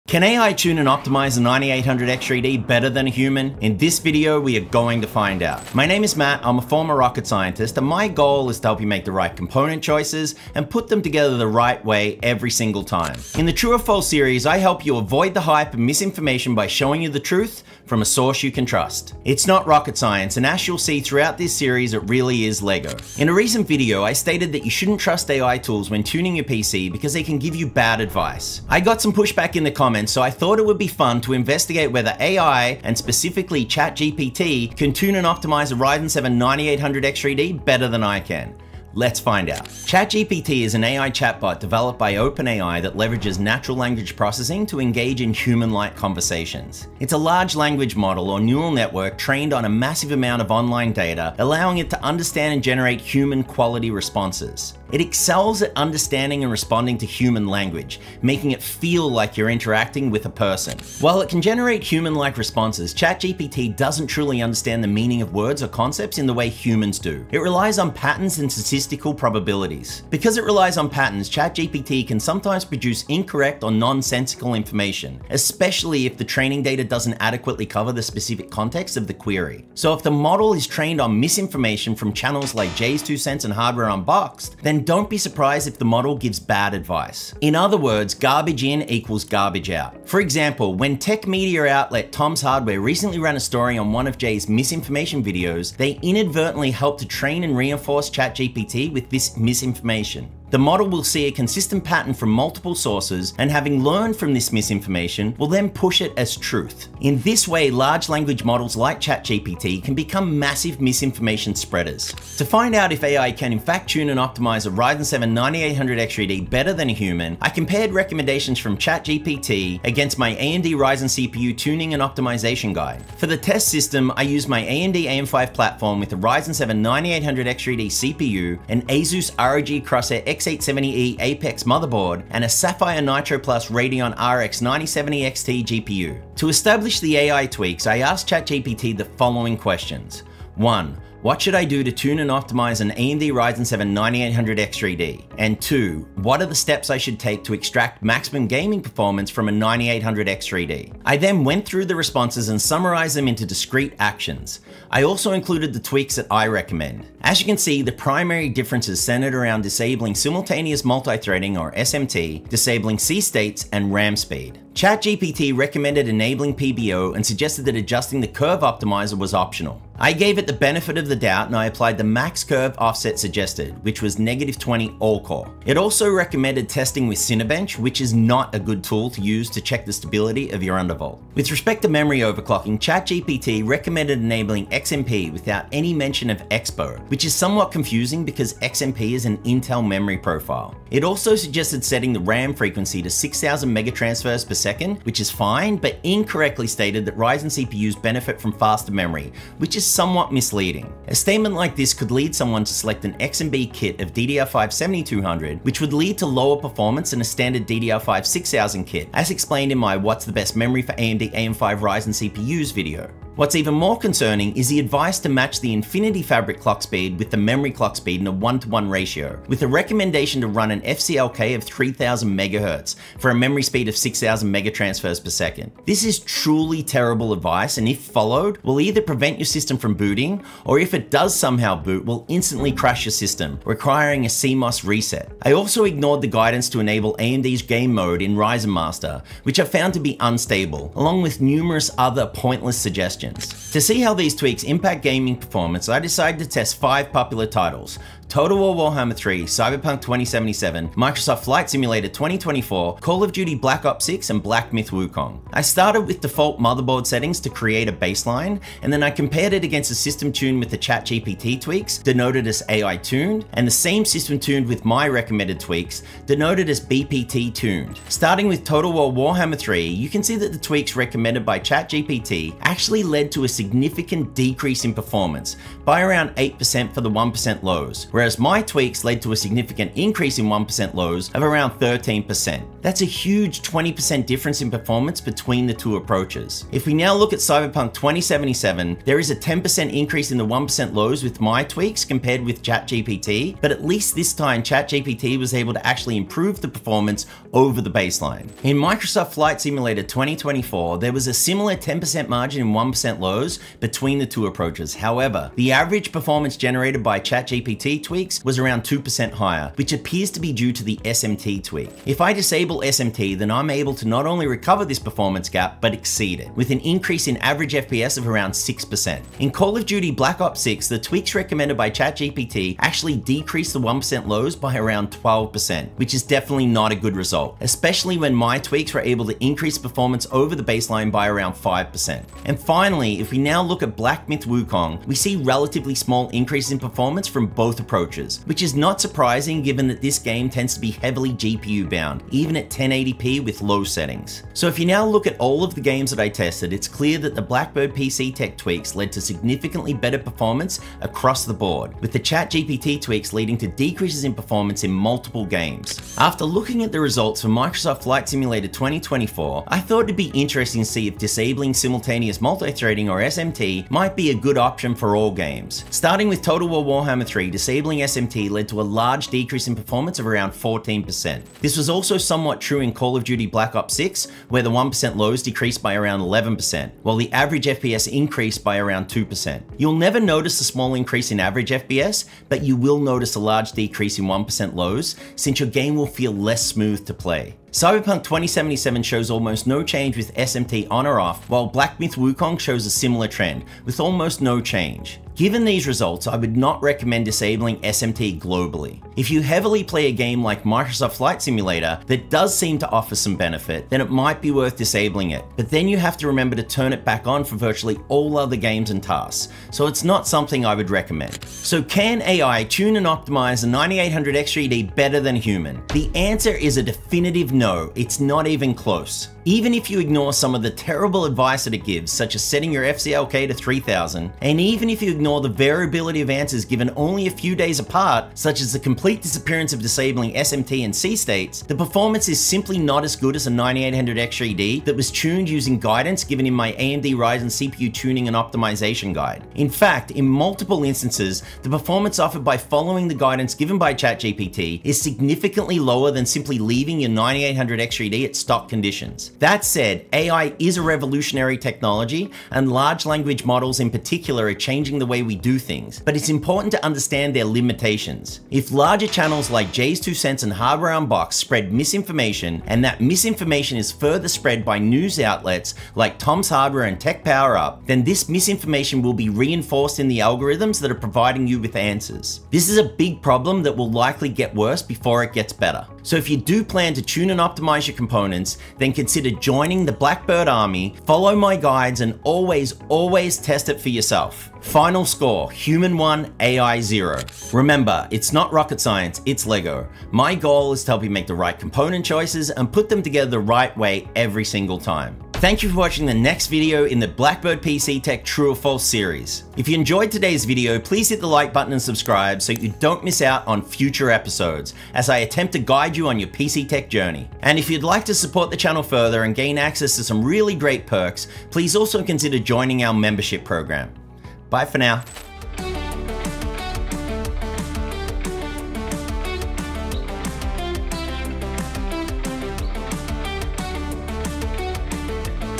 Can AI Tune A Ryzen 7 9800X3D CPU Better Than Me?
Can AI tune and optimize a Ryzen 7 9800X3D better than a human? In this video, the next in our "True or False?" series, we are going to find out. In the "True or False" series, I help you avoid the hype and misinformation, by showing you the truth, from a source you can trust.
AMD Ryzen 7 9800X3D
Asus ROG Crosshair X870E Apex
Sapphire Nitro+ Radeon RX 9070 XT
Title: Can AI Tune A Ryzen 7 9800X3D CPU Better Than Me?
Chapters:
00:00 - Introduction
00:23 - Question: Can AI Tune & Optimize a 9800X3D Better Than Me?
00:39 - What Is ChatGPT?
01:00 - What Are Some Potential Issues?
01:52 - The Test Setup
02:30 - The Recommended Tweaks
04:03 - The Gaming Benchmarks
05:56 - Should You Turn SMT On or Off?
06:51 - The Verdict: Can AI Tune & Optimize a 9800X3D Better Than Me?
08:07 - Wrap-Up